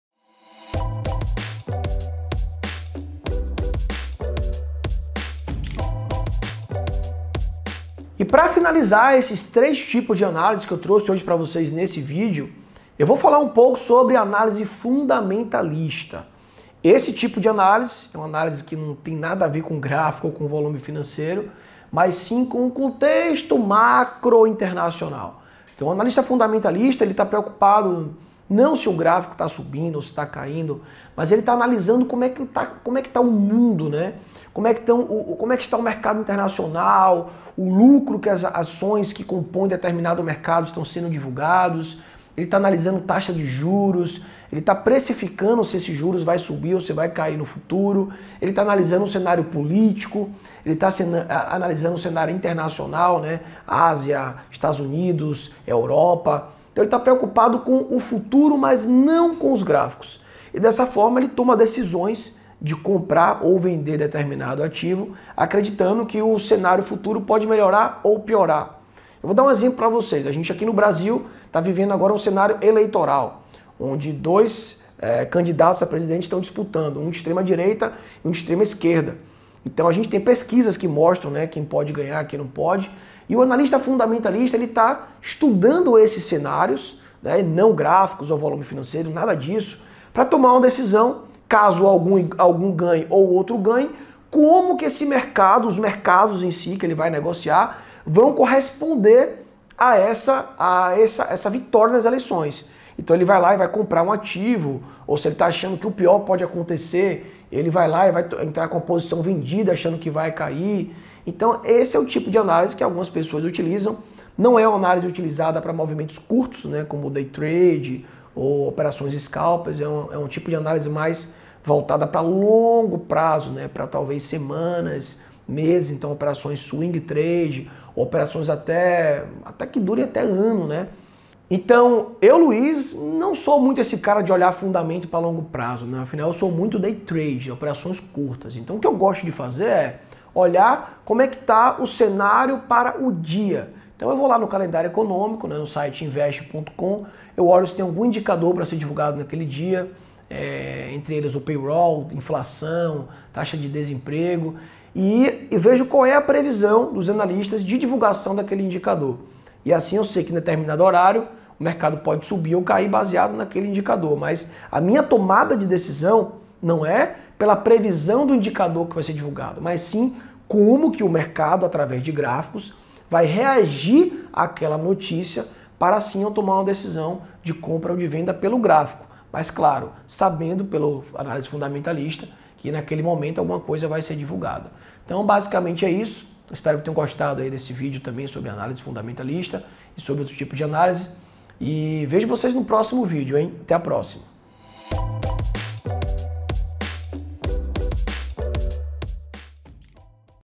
FXGlobe: Tipos De Análises Do Mercado Financeiro (3/3) 💹🏆
Qualquer trader profissional precisa se basear em algo para fazer suas operações e uma das mais conhecidas formas de analisar o mercado e assim tomar decisões profissionais de compra ou venda.

Neste vídeo fala sobre a importancia da análise fundamentalista.

Nesse tipo de análise o trader não deve focar nos gráficos e sim no estudos dos fundamentos, se um determinado ativo está caro ou barato em função do contexto que aquele ativo se encontra, se for uma ação por exemplo, se o valor de mercado que ela se encontra está de acordo com o lucro que a mesma apresentou ou irá apresentar no proximo resultado.

Esse é um tipo de analise que ajuda o trader na tomada de decisão de médio/longo prazo.

** JUNTA-TE À NOSSA COMUNIDADE PROFISSIONAL DE COMÉRCIO  **